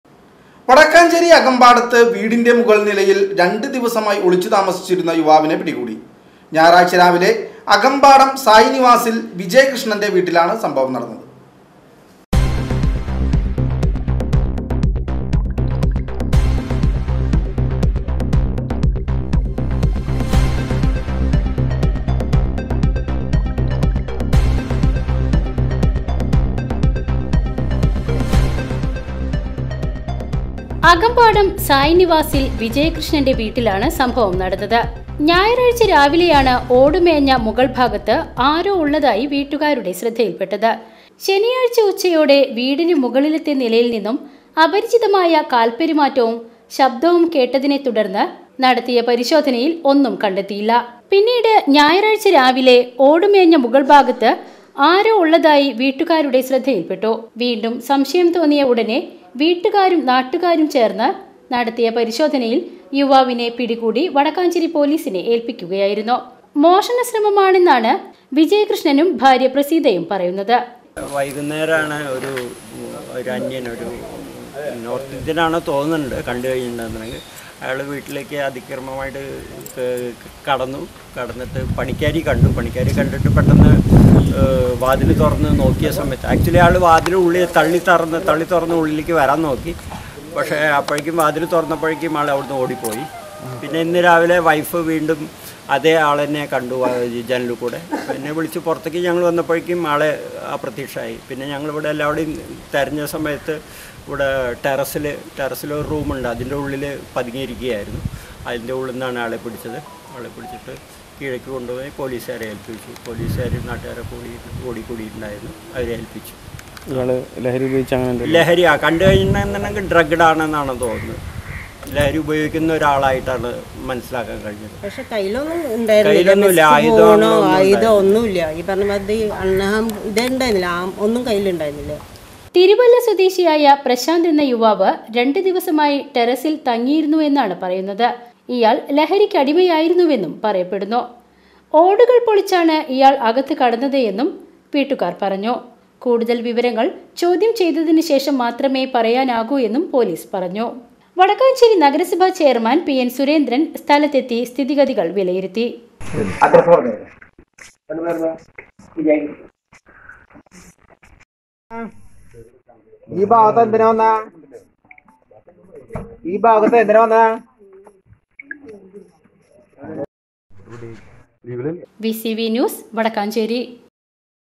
തിരുവല്ല സ്വദേശി പ്രശാന്ത് ആണ് വീടിനു മുകളിൽ ഒളിച്ച് താമസിച്ചിരുന്നത്; മോഷണ ശ്രമമെന്ന് വീട്ടുക്കാർ
# UPDATES
VCV NEWS

Wadakkanchery, Thrissur